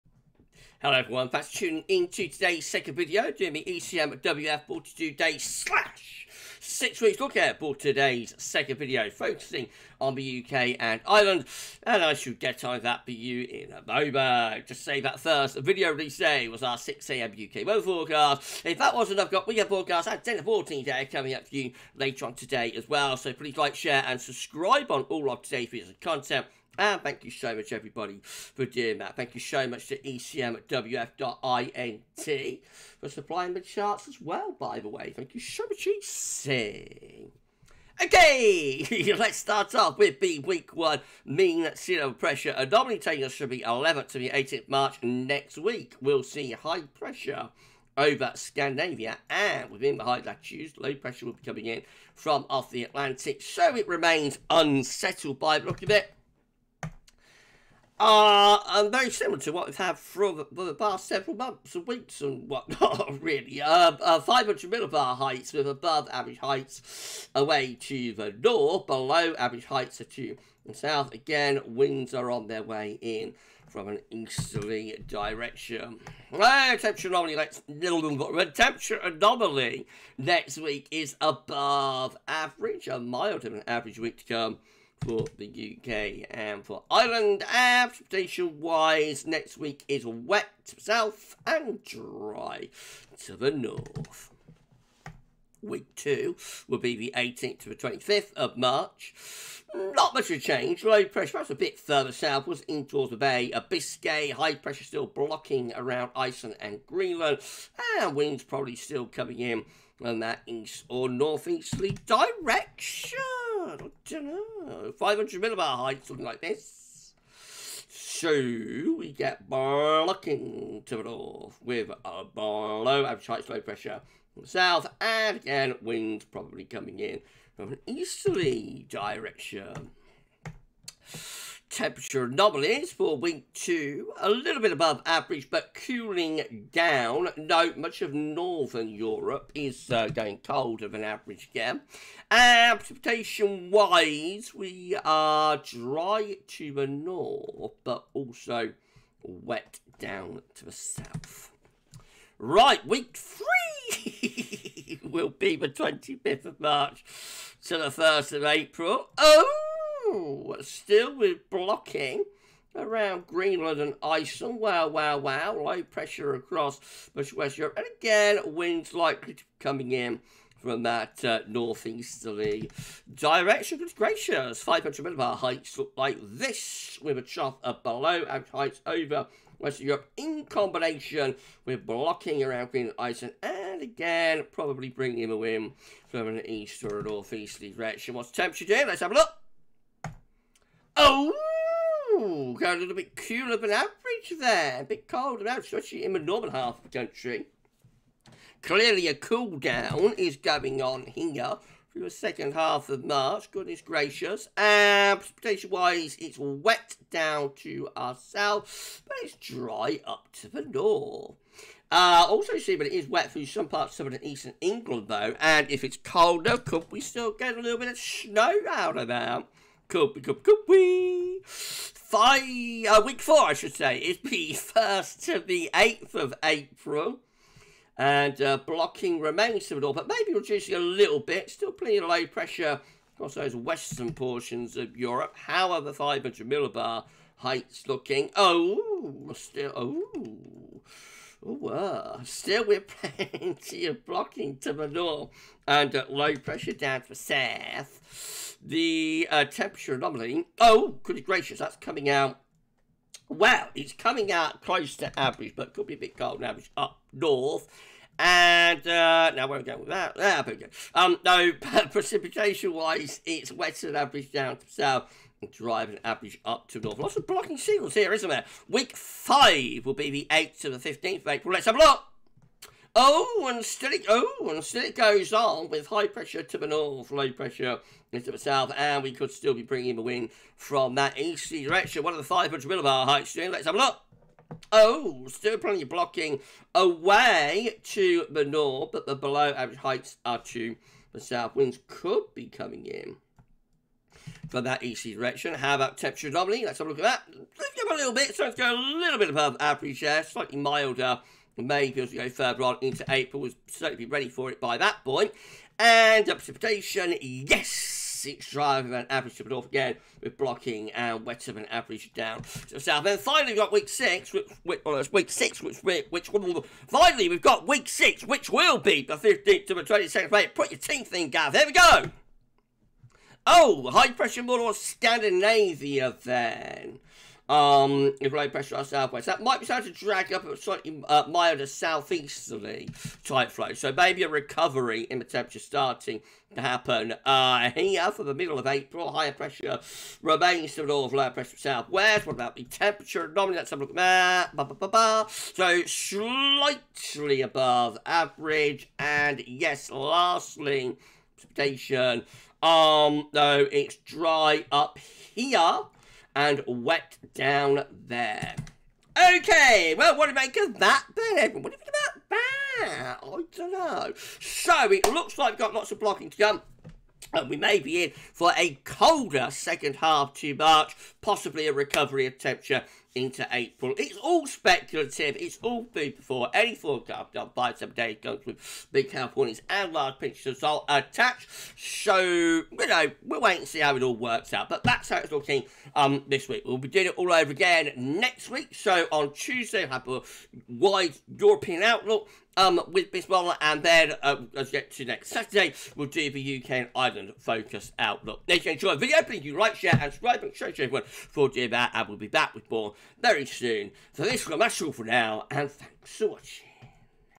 ECMWF 42 Day Forecast: Cold For Late March???
Welcome to this weeks extended ECMWF 46 day forecast for the UK and Ireland.

This video forecast from the extended ECMWF model takes us from 11th March to 22nd April and we're looking to see if northern blocking could deliver a cold late March?

💡  Video Credits: 💡

 💡 GWV Links: 💡